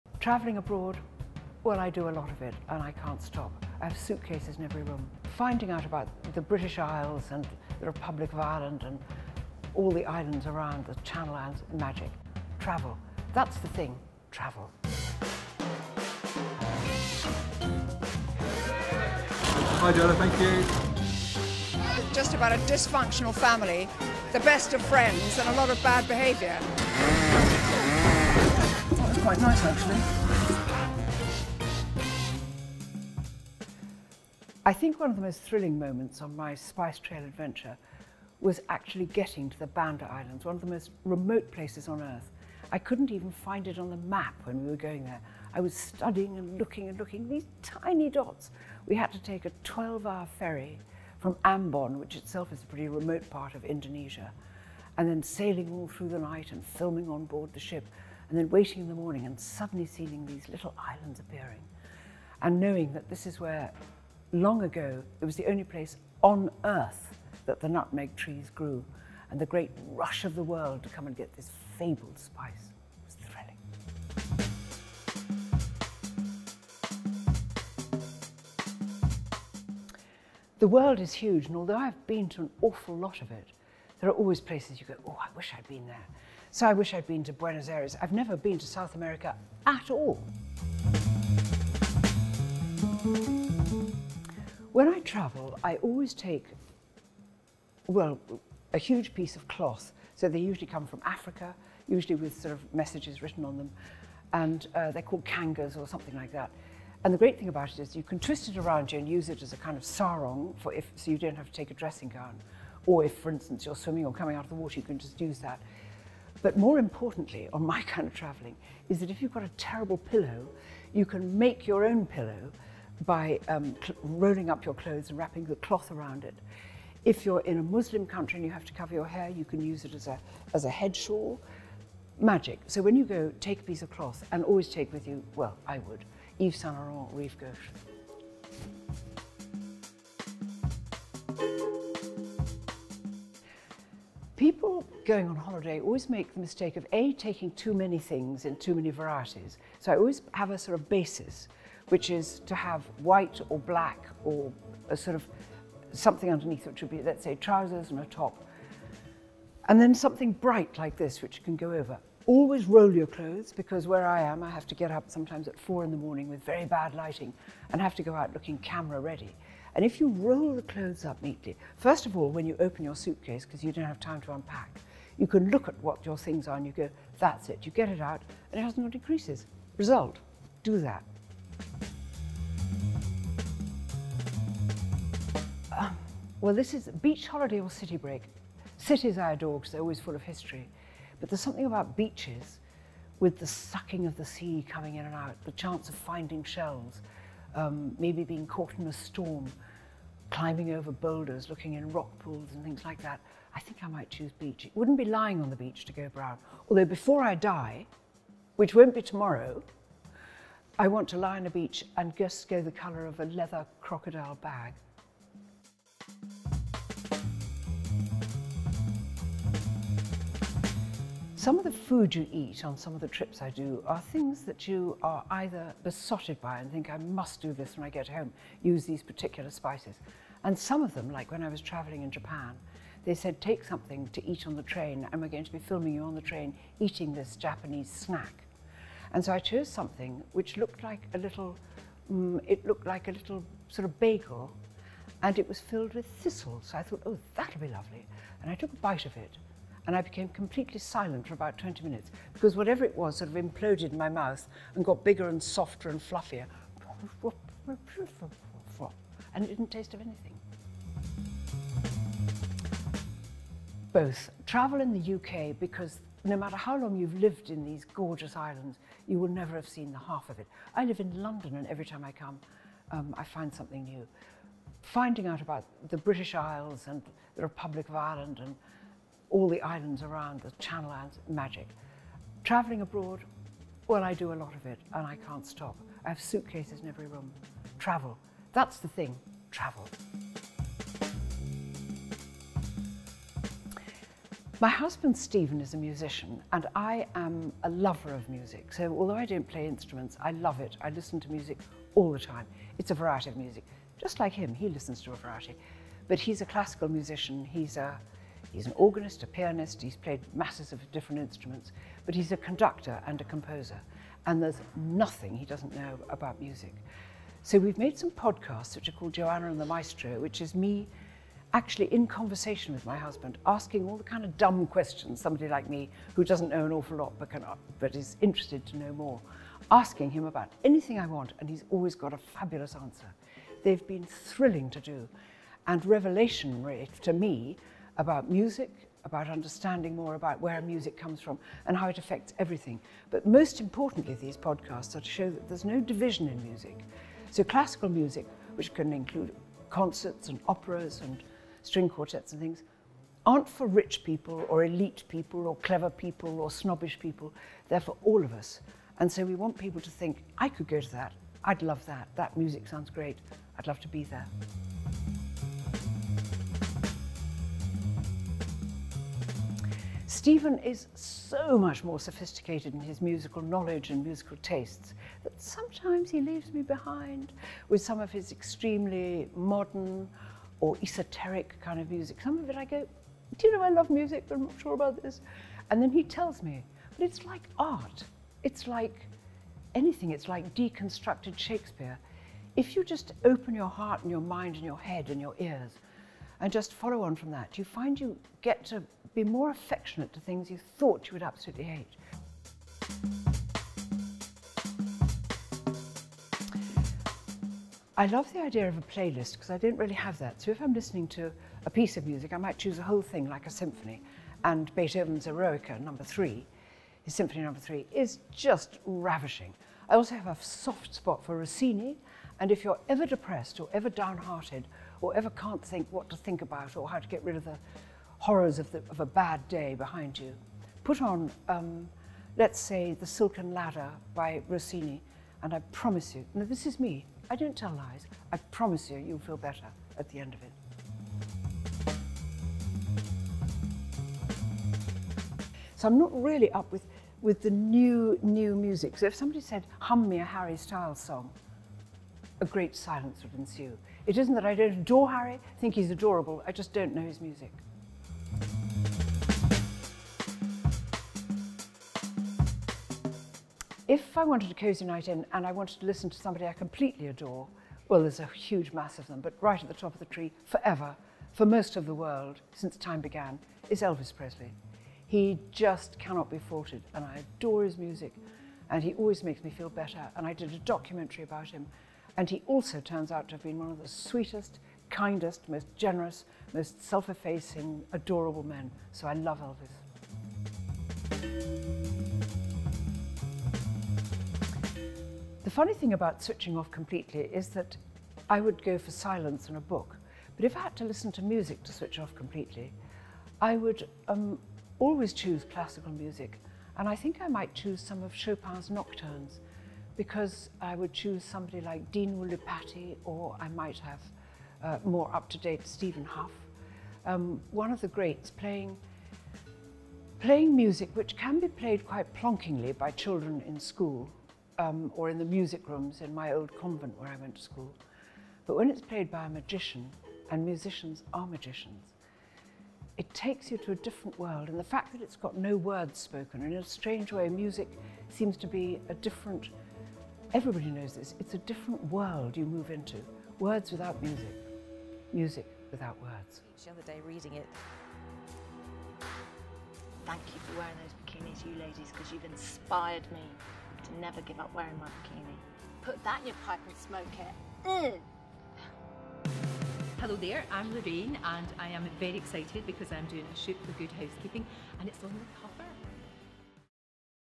Dame Joanna Lumley Can't Stop Travelling 'I Have Suitcases In Every Room' | Good Housekeeping UK
When it comes to travelling in Absolutely Fabulous style, look no further than the advice of Dame Joanna Lumley! Good Housekeeping chatted to the actor, activist and presenter about her Silk Road adventure, her new podcast with husband, conductor Stephen Barlow, and much more!

A lover of travel, Dame Joanna Lumley has more incredible travel stories and advice than you might think, from eating thistles in Japan and travelling to the most remote Indonesian islands in search of nutmeg, to what to always pack for every adventure

She’s also a passionate music fan, thanks to husband, conductor Stephen Barlow. Here, Joanna chats all things music, including her love of classical music and her favourite composers, and the fact she doesn’t know any of Harry Styles’ songs!

Watch the interview to learn about the passions of Dame Joanna Lumley, and find the full article

Head to our site for all the other food advice you might need